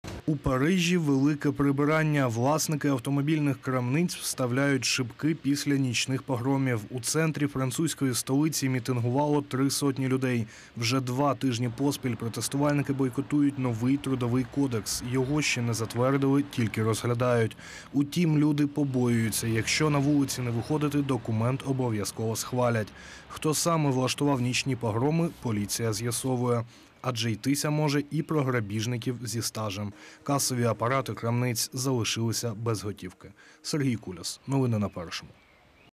У Парижі власники автомобільних крамниць вставляють шибки після нічних погромів
У центрі французької столиці мітингувало три сотні людей. Вже два тижні поспіль протестувальники бойкотують новий трудовий кодекс. Його ще не затвердили, тільки розглядають. Утім, люди побоюються, якщо на вулиці не виходити, документ обов'язково схвалять. Хто саме влаштував нічні погроми – поліція з'ясовує. Адже йтися може і про грабіжників зі стажем. Касові апарати крамниць залишилися без готівки.